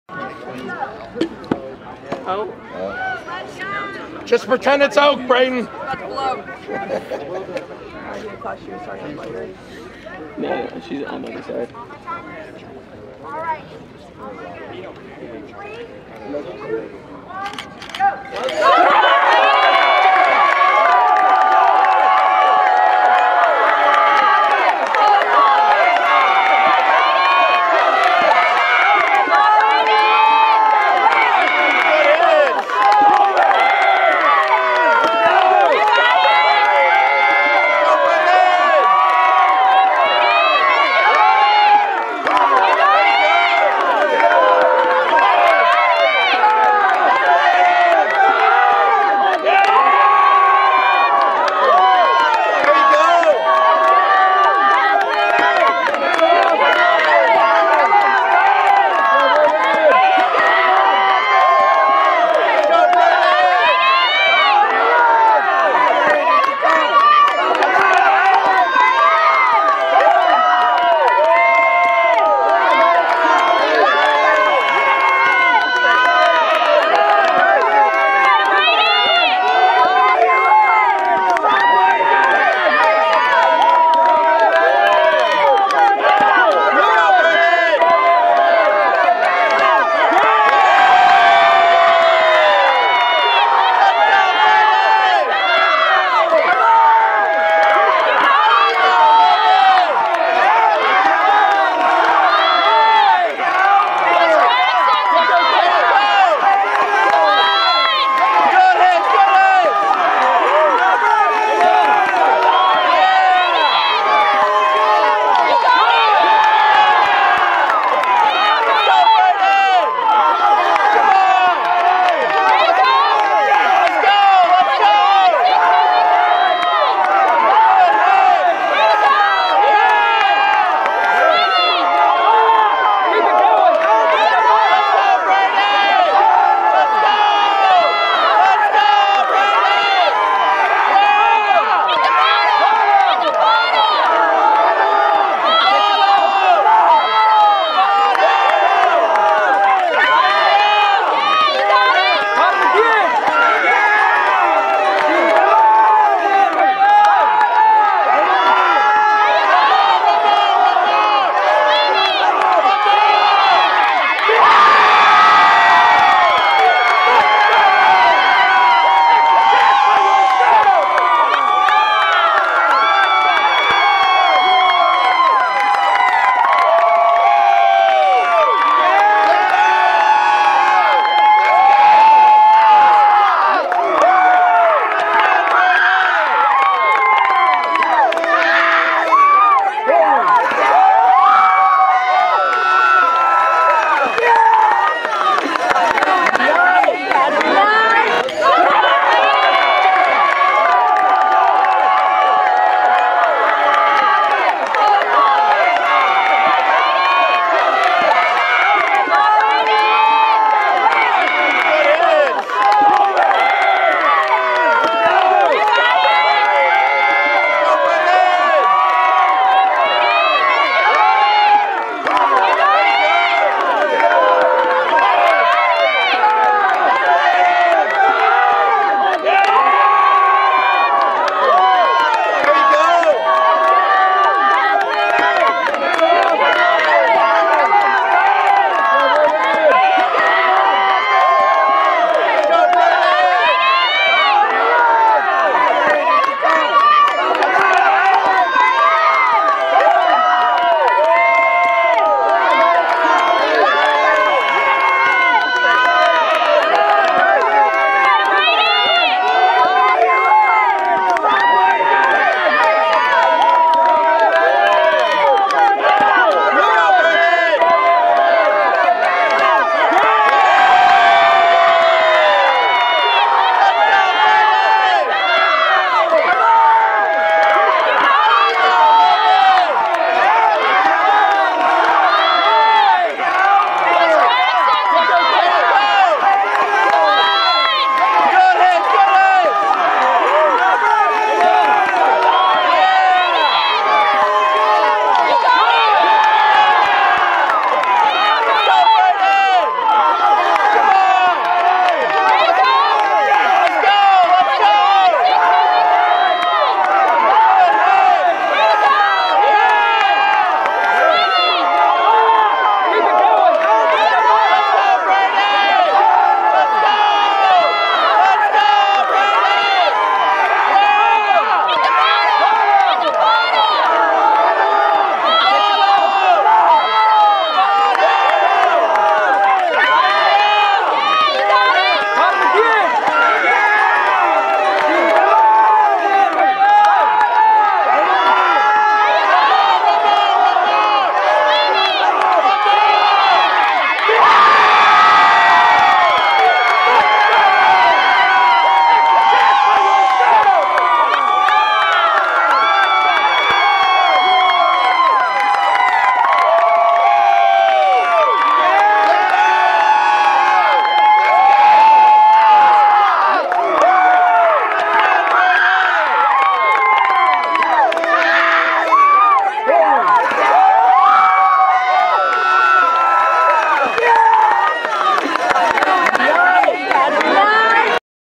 Women’s Log Chopping - 2023 Southern Forestry Conclave
SFA Sylvan Brayden Gill won the inaugural event and thus set the record at the 2023 Southern Forestry Conclave hosted by Louisiana Tech in Ruston, Louisiana. Brayden chopped an 8 x 8 sweetgum cant in only 2 minutes 26.80 seconds, becoming one of only three women to finish, and the record holder.
Video by Kolby Perrin. Drone footage by Tori Williams.